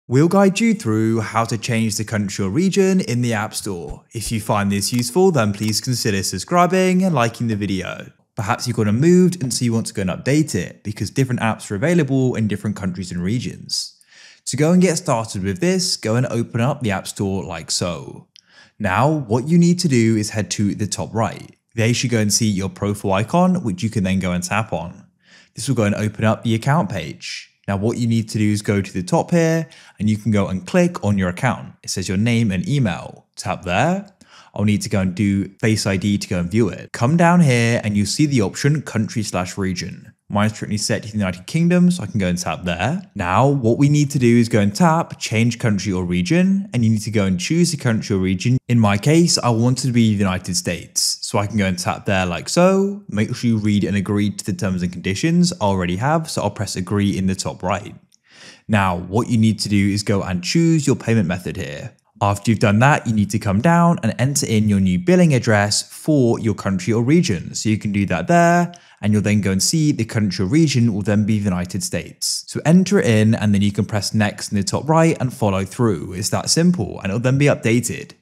How To Change Country In App Store - Full Guide
Learn how to change country or region in app store in this video.

GuideRealm